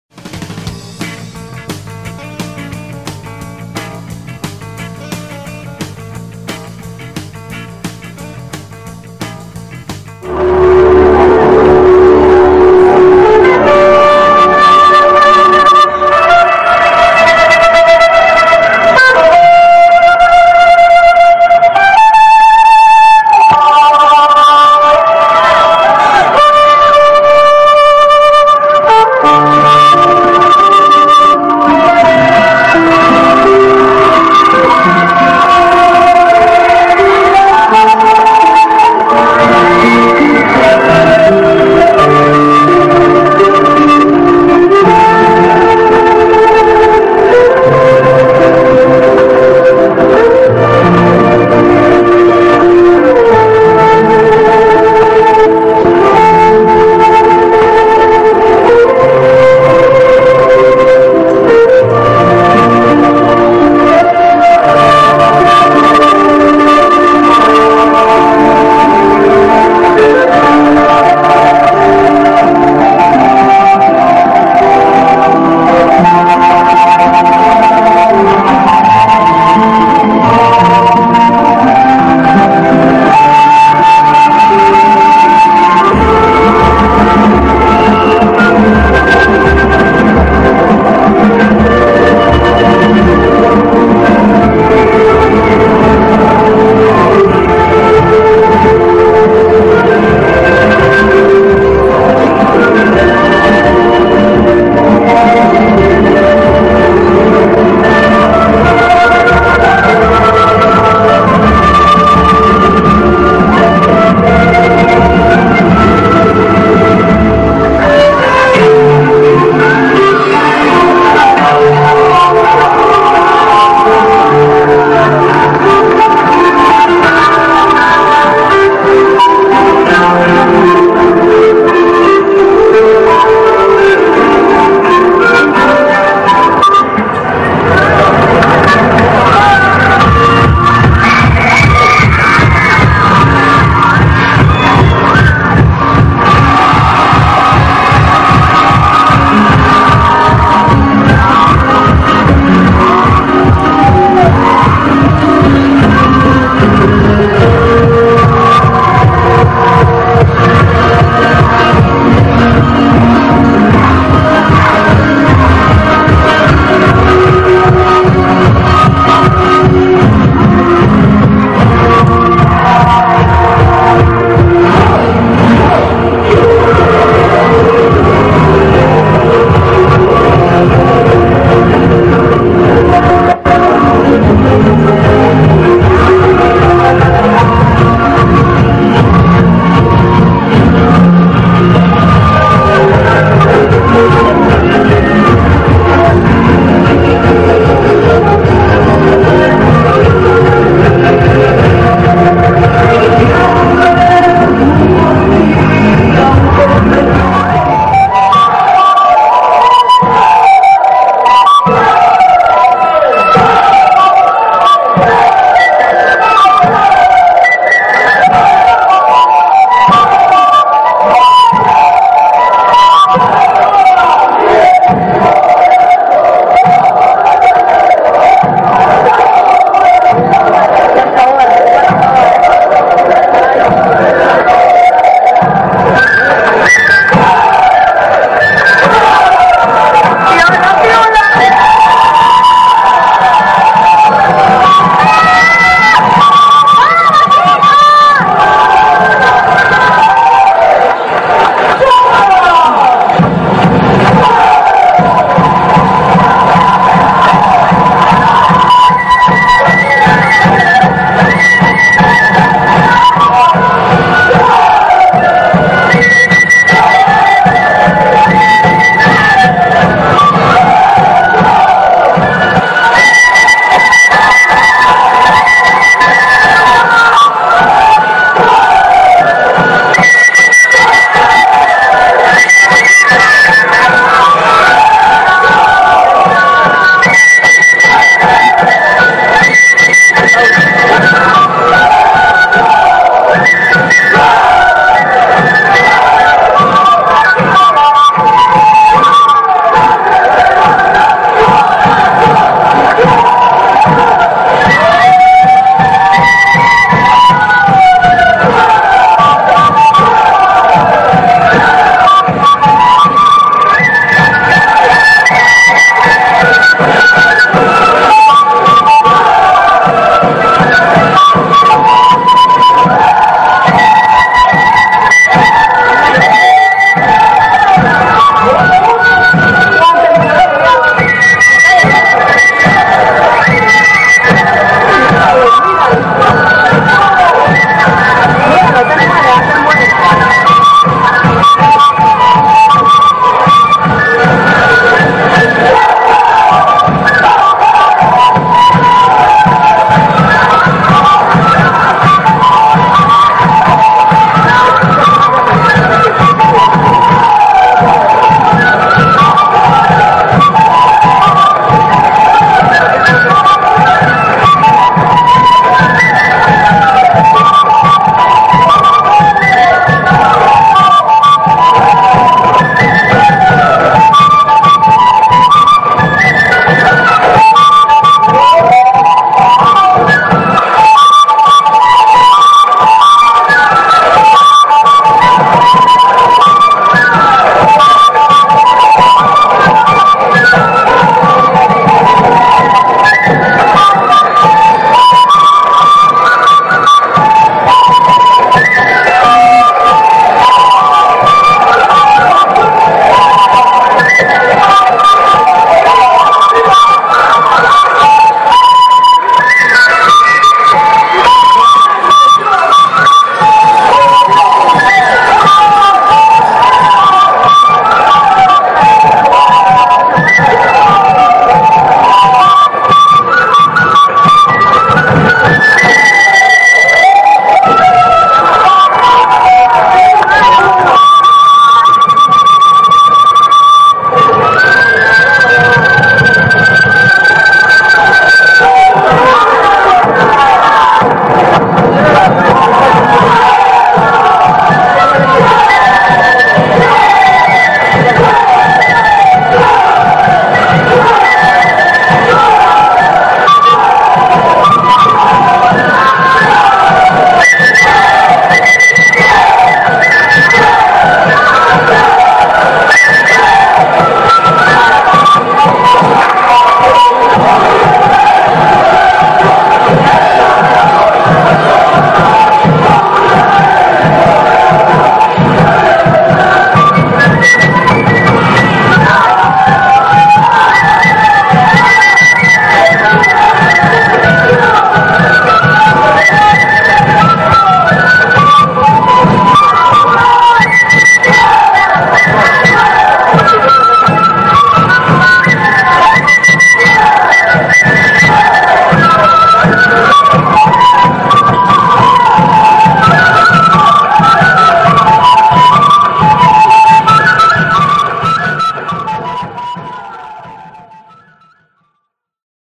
Participación del grupo de danza dominguina en el Concurso "Todas las Sangres"
Colegio Santo Domingo, el Apóstol
Formando Líderes que el Perú Necesita
Av. La Paz N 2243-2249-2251